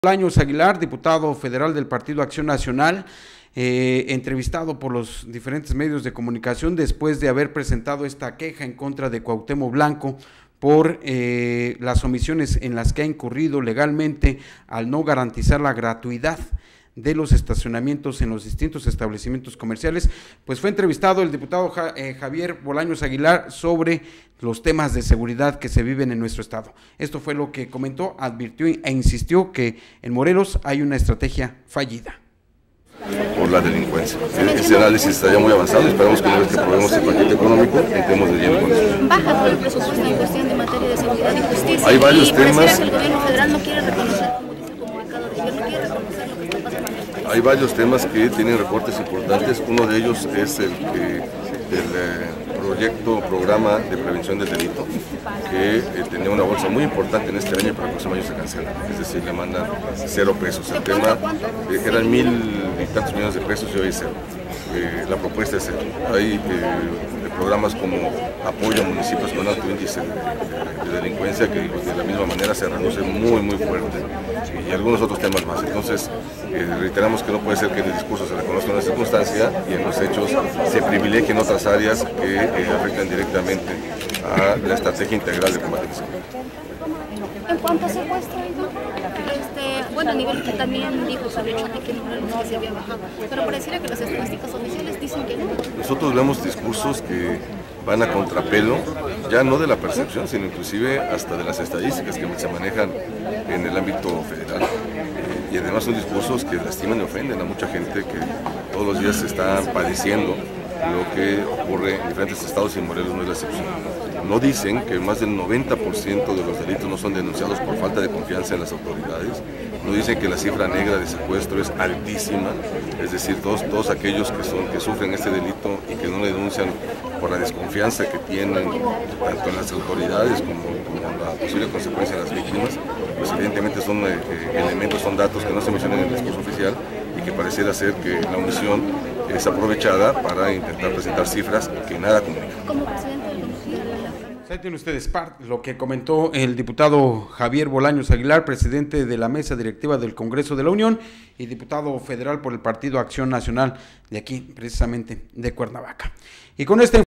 Ofensiva la inseguridad que existe en Morelos: Javier Bolaños
El 90% de los secuestros no se denuncian por temor; esa es la lista negra que oculta el gobierno